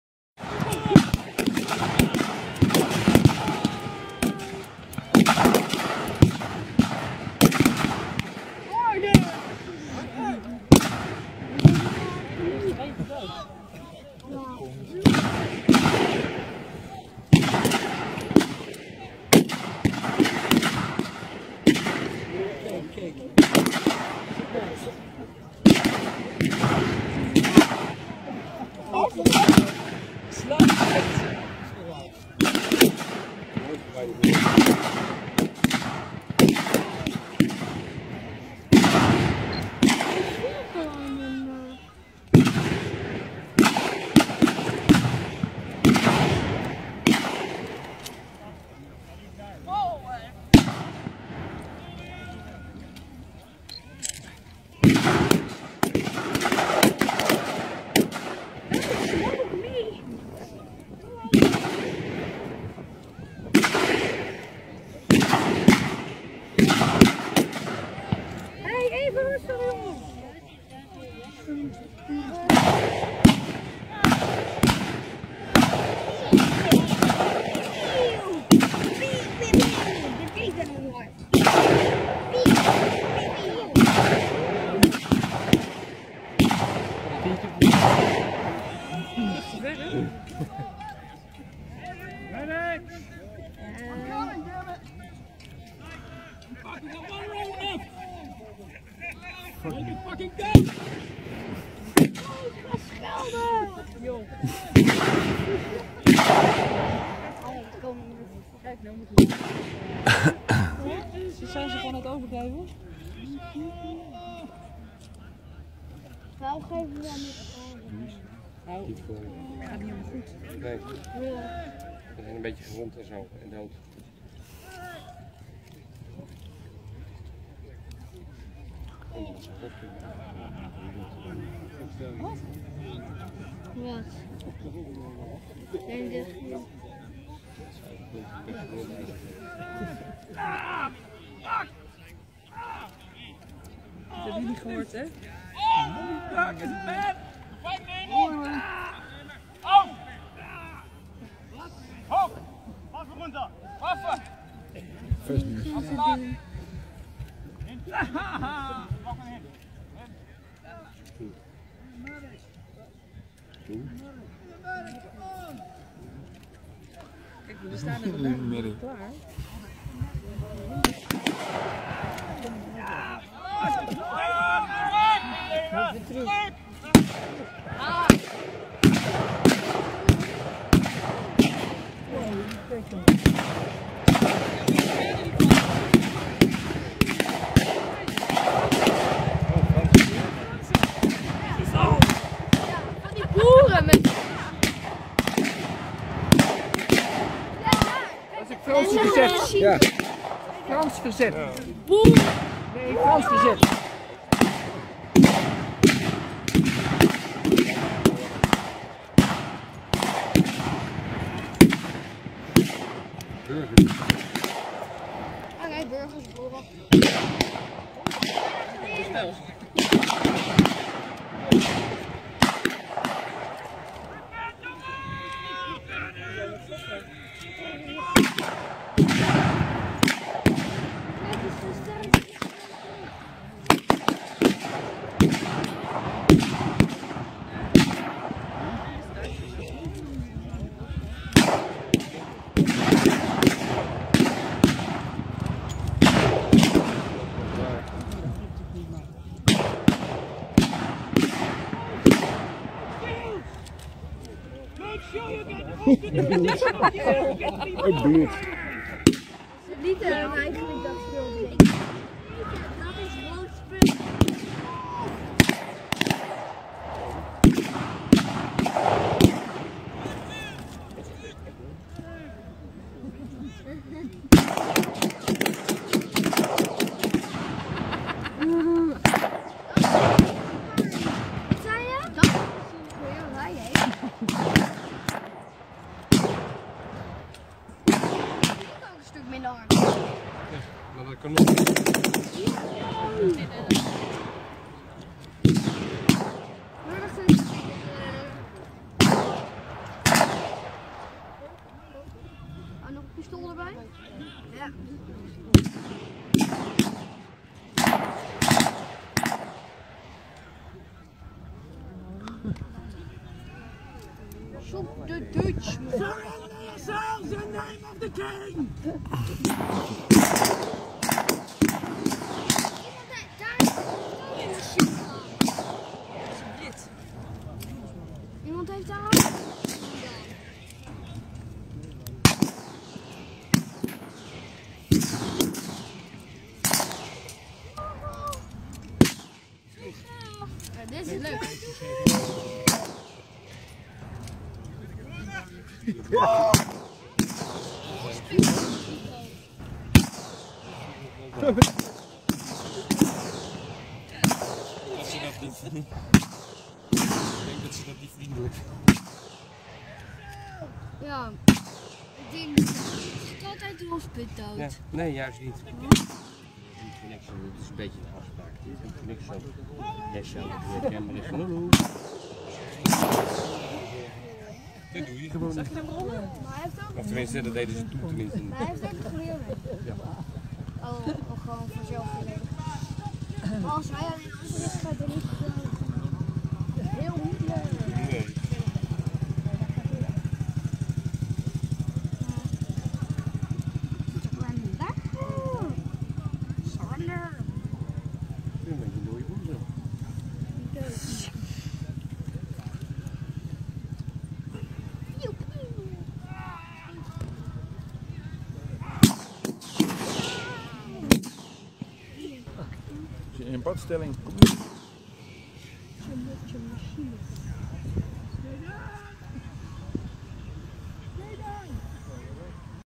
Tweede Wereldoorlog Historisch Festijn Vorden
Een aantal re-enactmentgroepen voeren "de bevrijding van Vorden" op tijdens het Historisch Festijn Vorden 2016. (Video van Jan Korenblek)